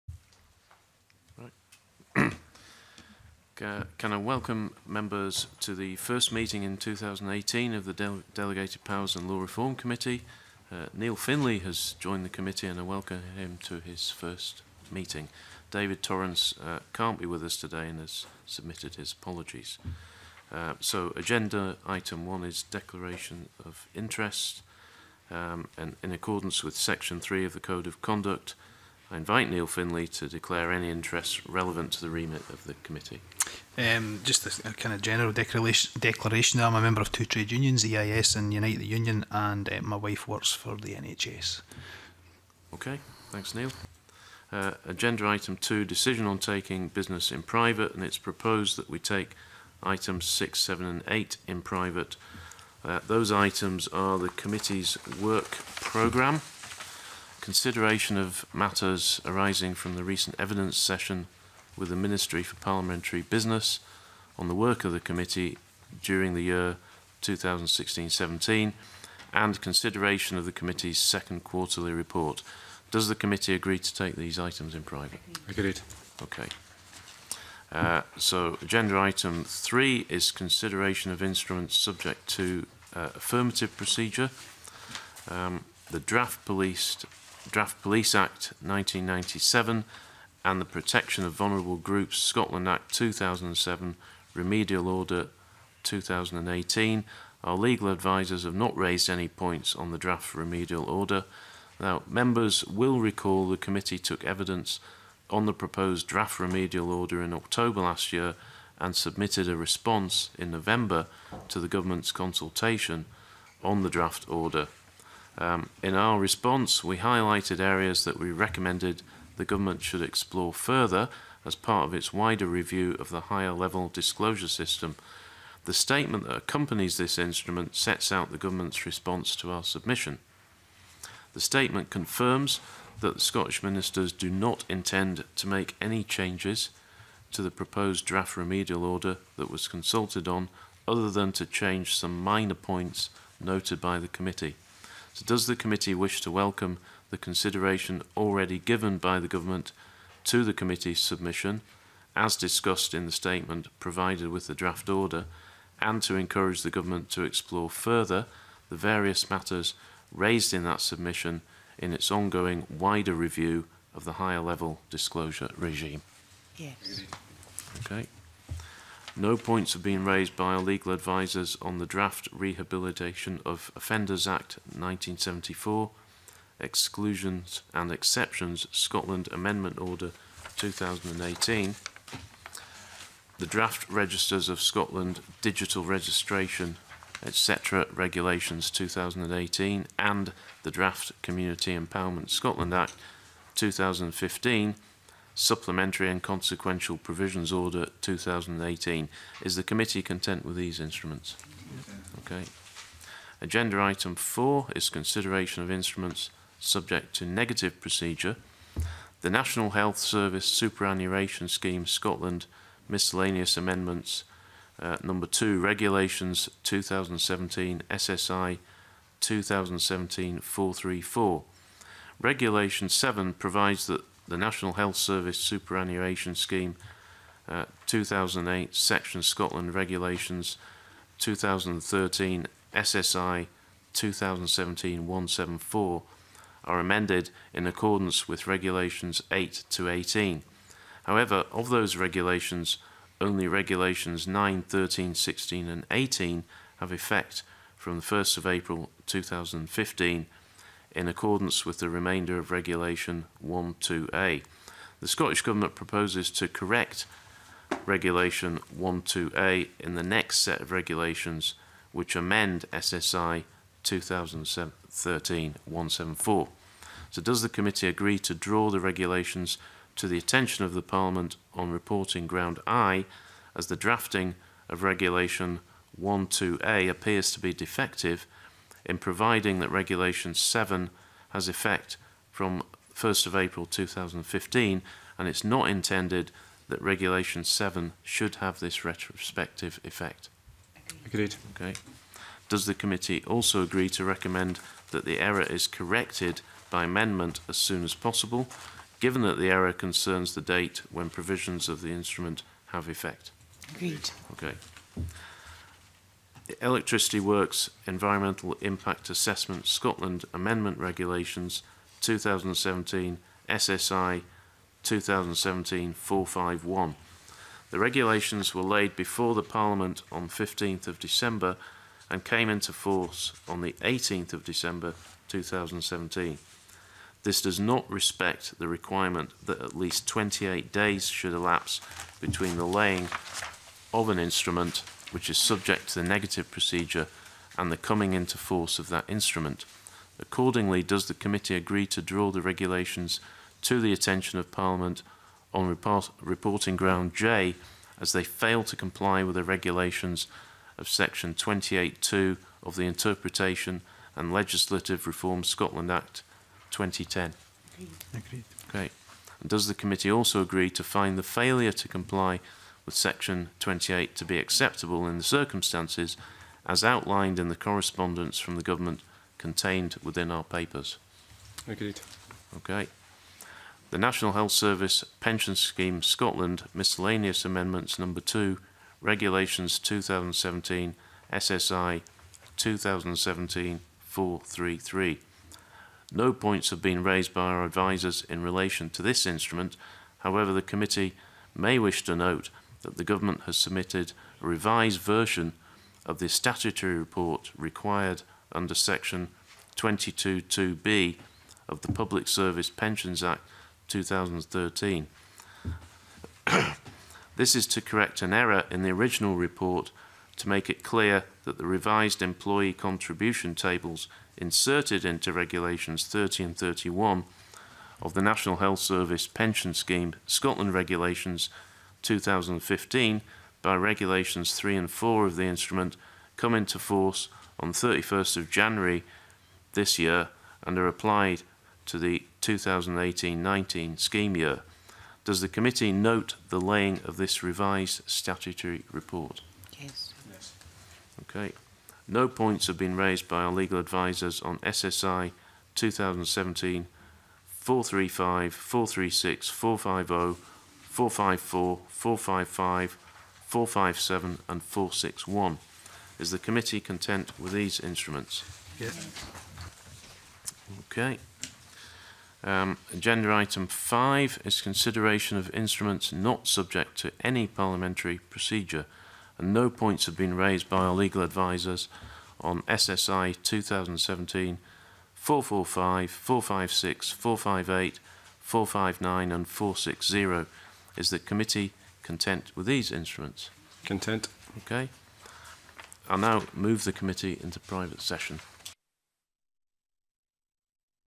Delegated Powers and Law Reform Committee - 16 January 2018
1. Declaration of interests: Neil Findlay will be invited to declare any relevant interests.

2. Decision on taking business in private: The Committee will decide whether to take items 6, 7 and 8 in private.

3. Instruments subject to affirmative procedure: The Committee will consider the following—
Police Act 1997 and the Protection of Vulnerable Groups (Scotland) Act 2007 Remedial Order 2018 [draft]; Rehabilitation of Offenders Act 1974 (Exclusions and Exceptions) (Scotland) Amendment Order 2018 [draft]; Registers of Scotland (Digital Registration, etc.) Regulations 2018 [draft]; Community Empowerment (Scotland) Act 2015 (Supplementary and Consequential Provisions) Order 2018 [draft].

4. Instruments subject to negative procedure: The Committee will consider the following—
National Health Service Superannuation Scheme (Scotland) (Miscellaneous Amendments) (No. 2) Regulations 2017 (SSI 2017/434); Electricity Works (Environmental Impact Assessment) (Scotland) Amendment Regulations 2017 (SSI 2017/451); National Health Service Pension Scheme (Scotland) (Miscellaneous Amendments) (No. 2) Regulations 2017 (SSI 2017/433); Firefighters’ Pension Scheme (Amendment and Transitional Provision) (Scotland) Regulations 2017 (SSI 2017/435); Universal Credit (Claims and Payments) (Scotland) Amendment Regulations 2017 (SSI 2017/436); Sea Fish (Prohibited Methods of Fishing) (Firth of Clyde) Order 2017 (SSI 2017/450); Teachers’ Pension Scheme (Scotland) (No. 2) Amendment Regulations 2017 (SSI 2017/454); Specified Crustaceans (Prohibition on Landing, Sale and Carriage) (Scotland) Order 2017 (SSI 2017/455); Allotments (Compensation) (Scotland) Regulations 2017 (SSI 2017/457); Forced Marriage etc. (Protection and Jurisdiction) (Scotland) Act 2011 (Relevant Third Party) Order 2017 (SSI 2017/461).

5. Instruments not subject to any parliamentary procedure: The Committee will consider the following—
Criminal Justice and Licensing (Scotland) Act 2010 (Commencement No. 14 and Saving Provision) Order 2017 (SSI 2017/445 (C.33)); Criminal Finances Act 2017 (Commencement) (Scotland) Regulations 2017 (SSI 2017/456 (C.34)); Community Empowerment (Scotland) Act 2015 (Commencement No. 10, Saving, Transitional and Transitory Provisions) Order 2017 (SSI 2017/458 (C.35)); Act of Sederunt (Summary Applications, Statutory Applications and Appeals etc. Rules Amendment (Transfer from Lands Tribunal for Scotland) 2017 (SSI 2017/459); Act of Sederunt (Summary Applications, Statutory Applications and Appeals etc. Rules Amendment (Drug Dealing Telecommunications Restriction Orders) 2017 (SSI 2017/460).

6. Work programme: The Committee will consider its work programme and further consider its approach to the European Union (Withdrawal) Bill.

7. Consideration of the work of the Committee during the parliamentary year 2016-17: The Committee will give further consideration to matters arising from the recent evidence session with the Minister for Parliamentary Business.

8. Report on instruments considered during the second quarter of the parliamentary year 2017-18: The Committee will consider a draft of its second quarterly report for the parliamentary year 2017-18.